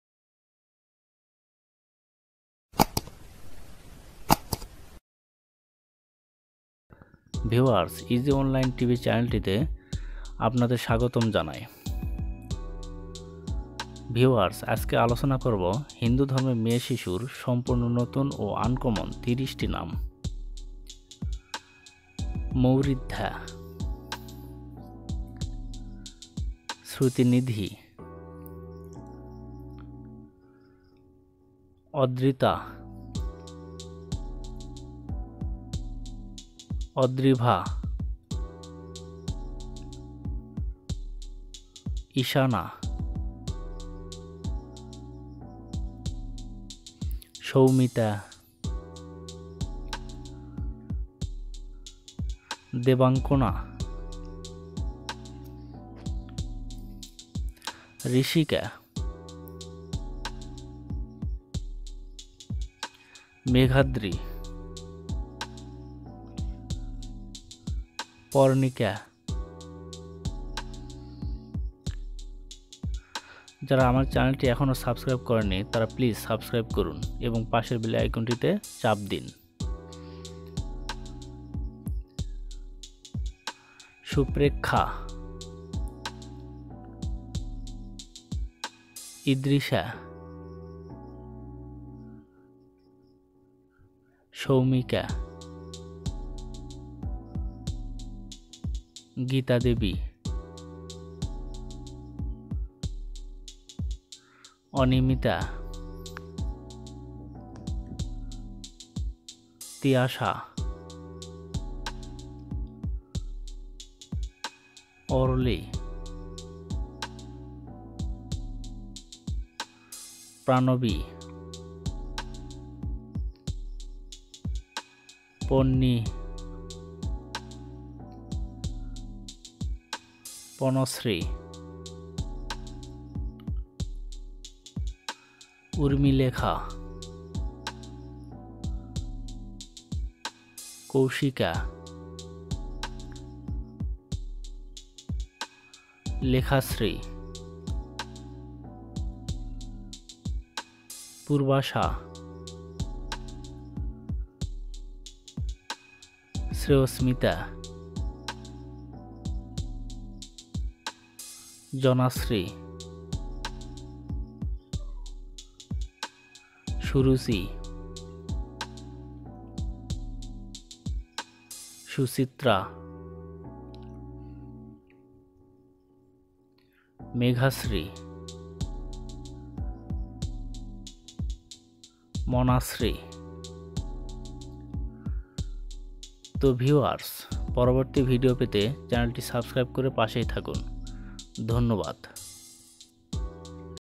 হিন্দু মেয়েদের আকর্ষণীয় নামের তালিকা | হিন্দু মেয়ে শিশুর সম্পূর্ণ নতুন ও আনকমন ৩০ টি নাম
হিন্দু মেয়েদের আকর্ষণীয় নামের তালিকা। হিন্দু মেয়ে শিশুর সম্পূর্ণ নতুন ও আনকমন নাম ৩০ টি।

হিন্দু মেয়ে শিশুর অক্ষর ভিত্তিক নামের তালিকাঃ

হিন্দু ছেলে শিশুর অক্ষর ভিত্তিক নামের তালিকাঃ